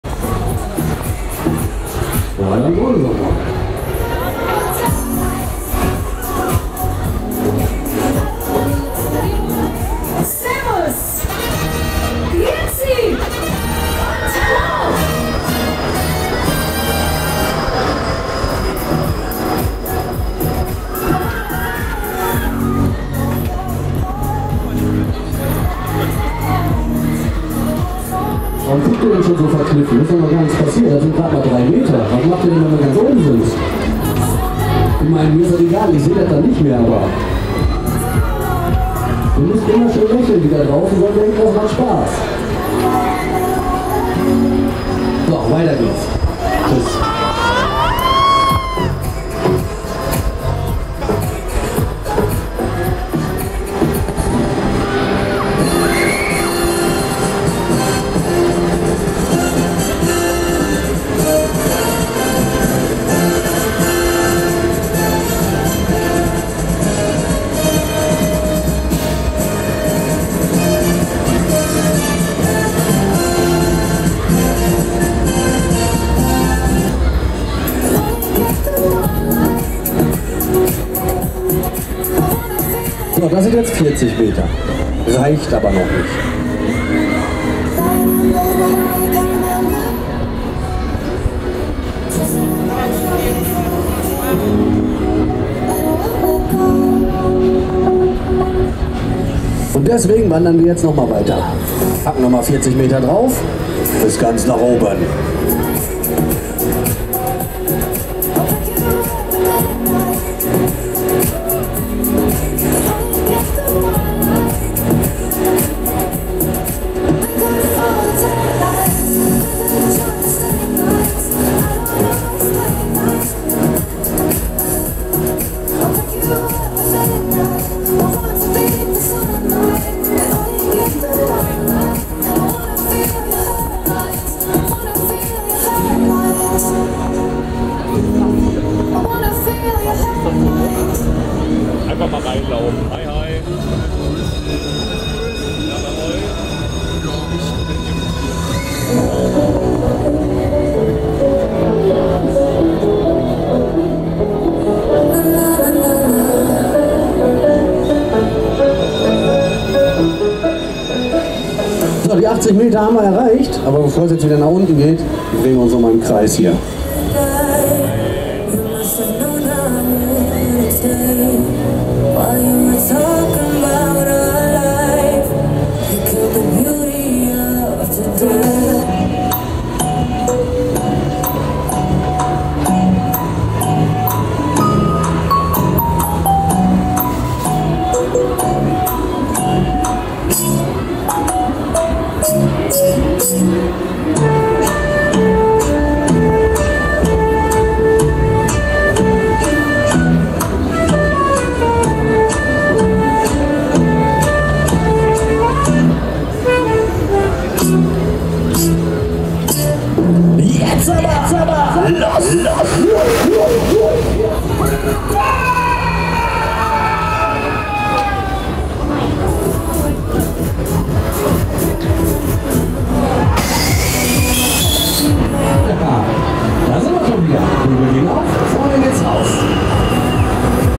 Fortress Tower - Goetzke (Offride) Video Frühjahrs Send Münster 2023
Fortress Tower von Goetzke Offride Video vom Frühjahrs Send in Münster 2023. 80 Meter hoher Funtime Freefall Tower / Freifallturm Fortress Tower Volksfest / Kirmes Münster 2023.

Fahrpreis: 6,00€